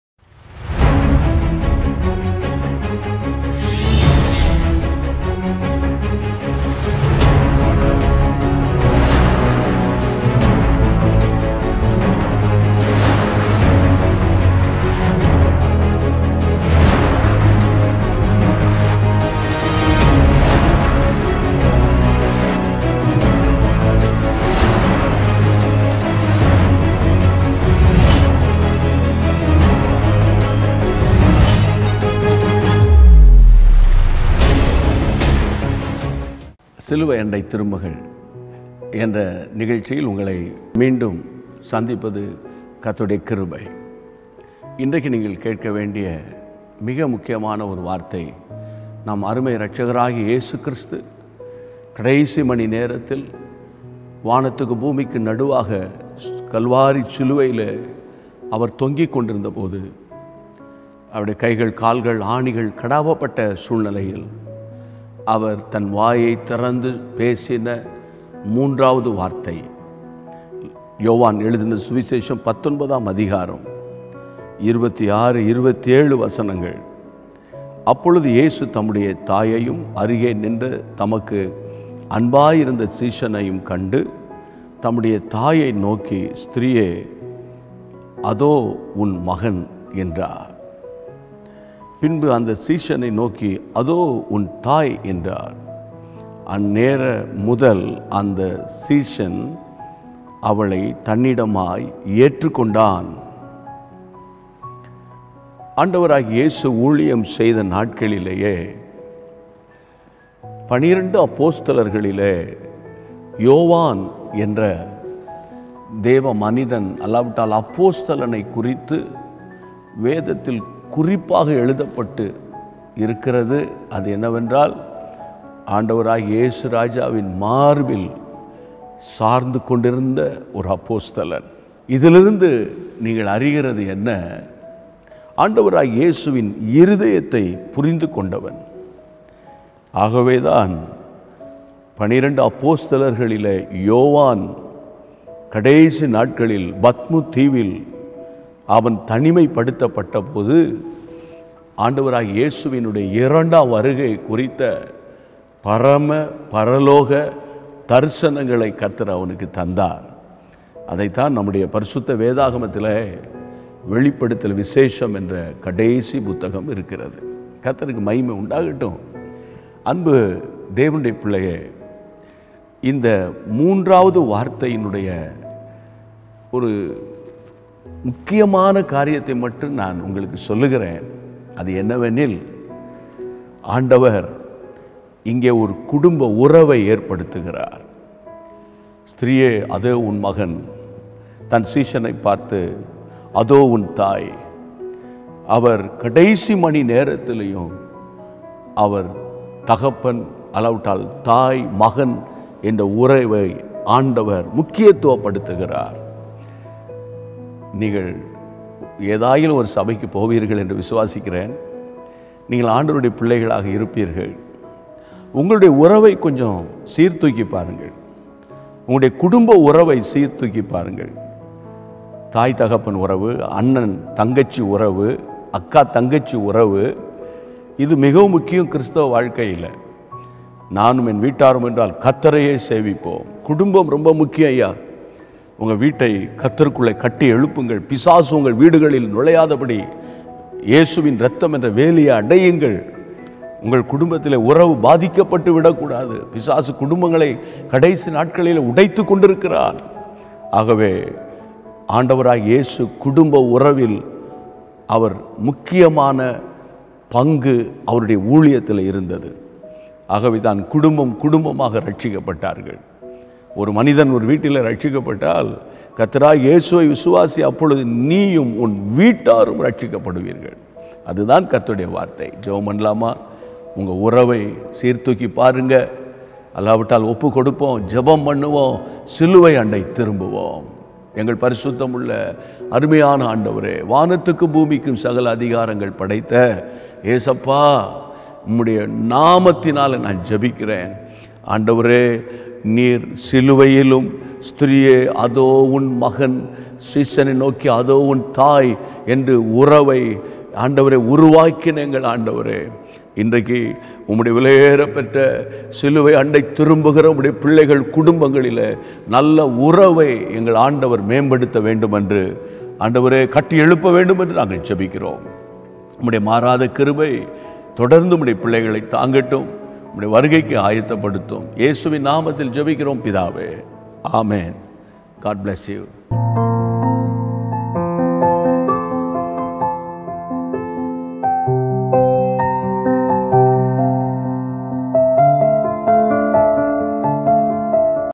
சிலுவையண்டை திரும்புங்கள் | Turn to the Cross| Day - 39 | Bro GPS. Robinson | lent special
Dear beloved Turn to the Cross is a special lent daily devotion program!
Bro GPS Robinson and Sis Christina Robinson will be sharing with you the truths about the cross which will transform lives and bring peace in families!!